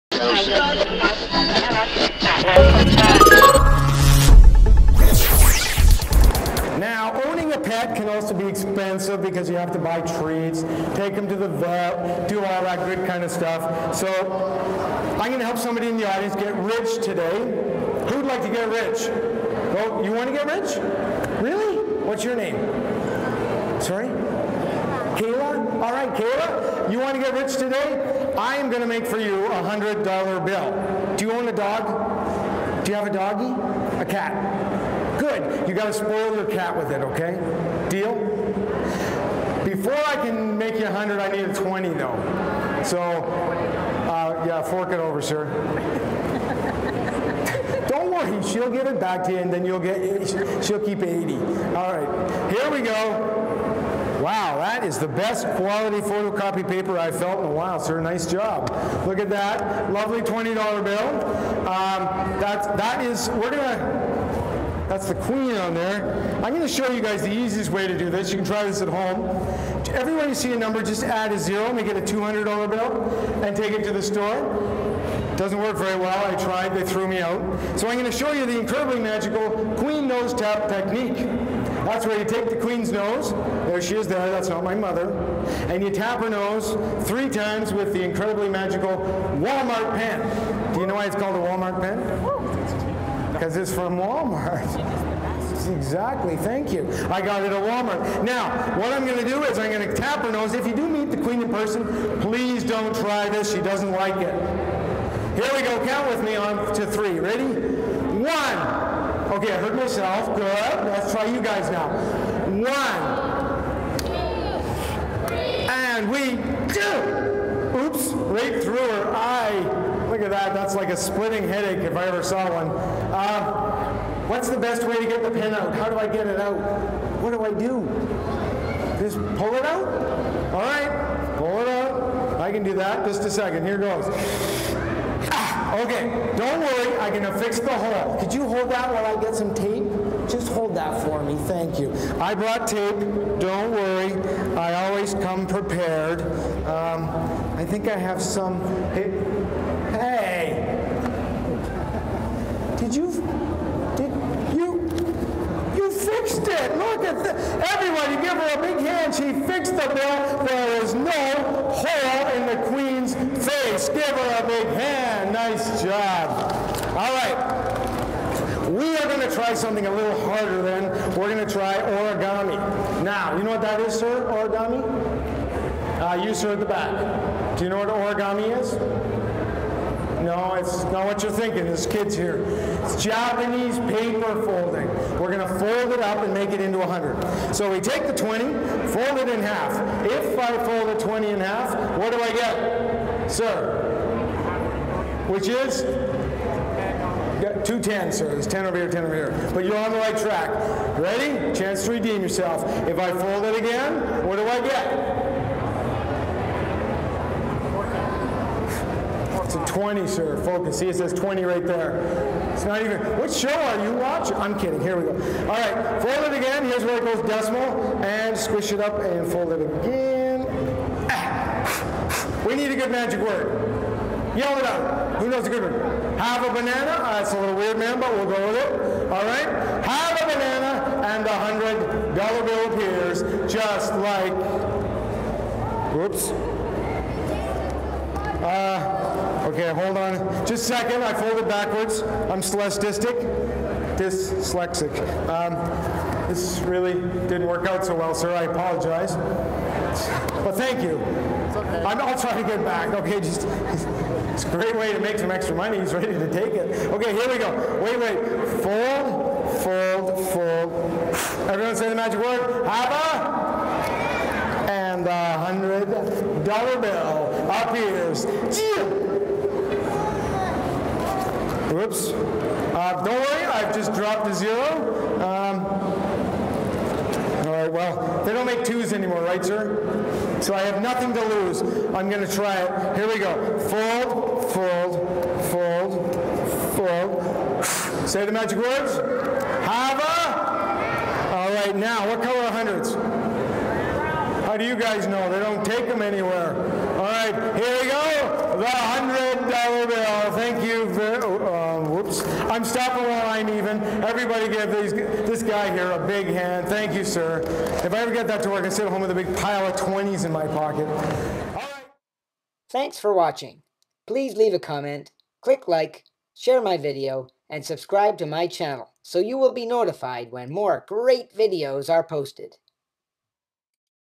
Ottawa Magician Magical Marvin & the $20 bill Trick!
Ottawa Magician Magical Marvin performs his amazing $20 bill trick at the SuperEx exhibition.  Another great video by Burke BunchTV!